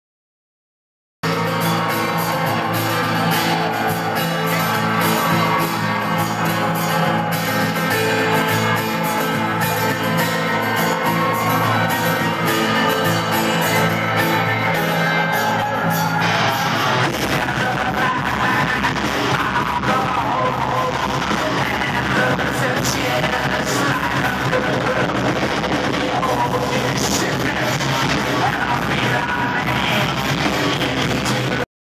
Rob zombie destroying the armory 10/16
A quick look at the set put on by zombie at the armory in Rochester ny. Zombie put on an awesome show..